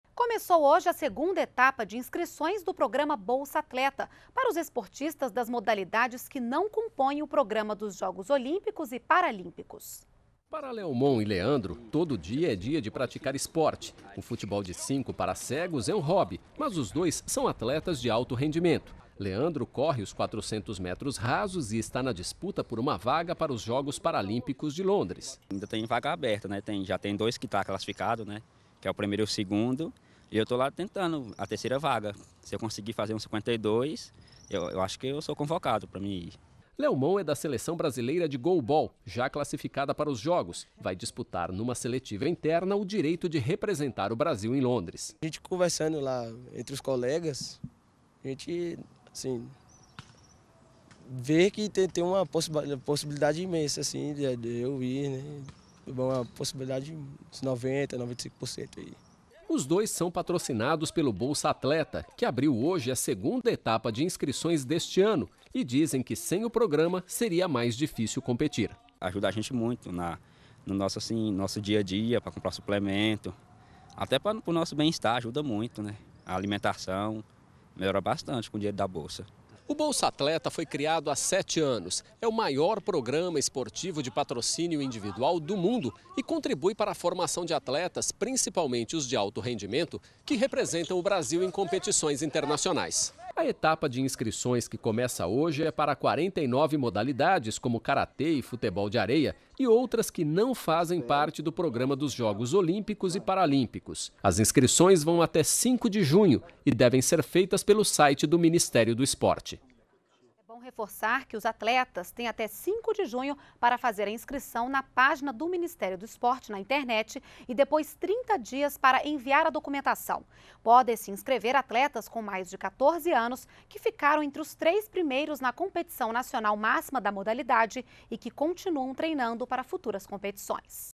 Abertas as inscrições para a 2ª etapa do programa Bolsa-Atleta
NBR NOTÍCIAS - 07.05.12: Estão abertas as inscrições para a 2ª etapa do programa Bolsa-Atleta. Nesta etapa, podem se inscrever os esportistas das modalidades que não compõem o programa dos Jogos Olímpicos e Paralímpicos. Para participar é preciso que os atletas tenham mais de 14 anos, que estejam entre os três primeiros na competição nacional máxima da modalidade e que estão treinando para futuras competições. Os atletas têm até 5 de junho para fazer a inscrição na página do Ministério do Esporte na internet e depois 30 dias para enviar a documentação.